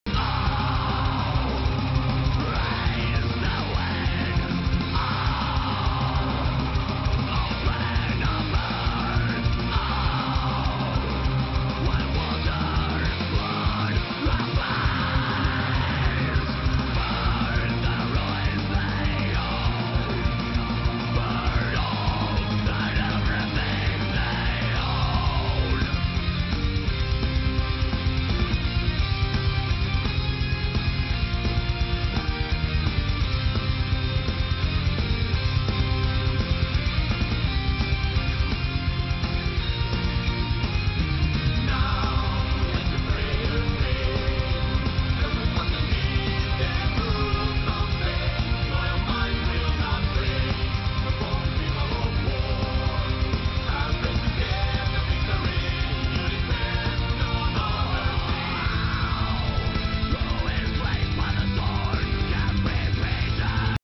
Preparation for BvB in Solitude [2/2] -DM vs USA-
Preparations for the big BvB war in Solitude, which I am uploading almost completely. Death Mental vs Pants